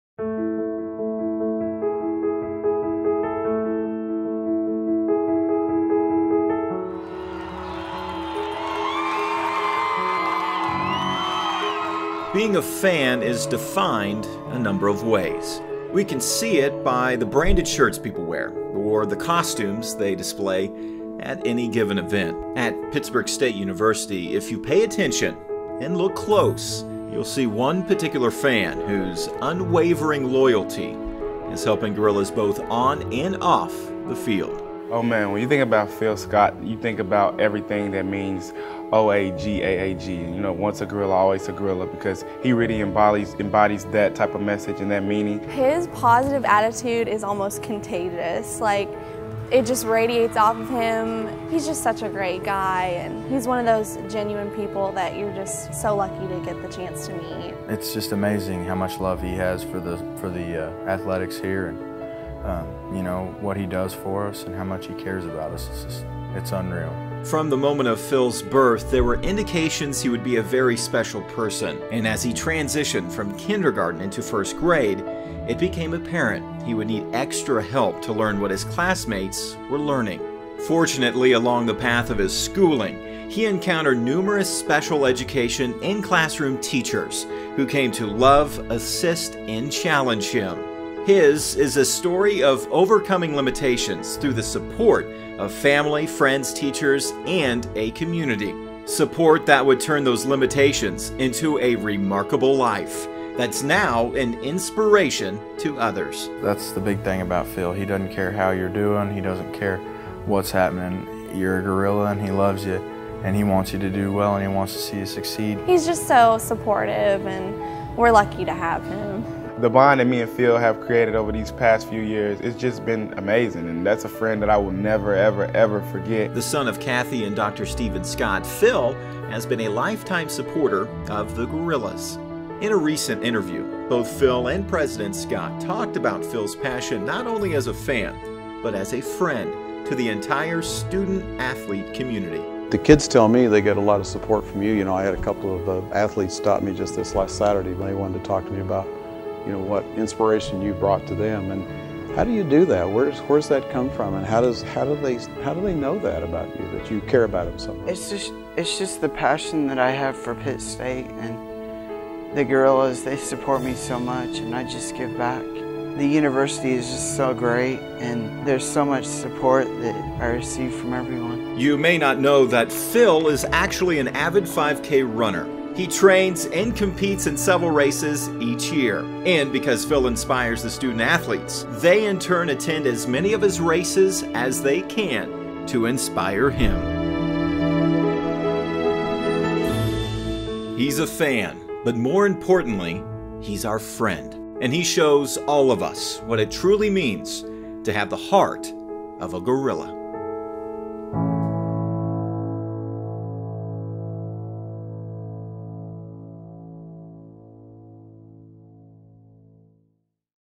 The Heart of a Gorilla - Pittsburg State University
Sometimes in life, we're blessed with the opportunity to meet certain people.  Phil Scott is definitely one of those people.

Pittsburg State University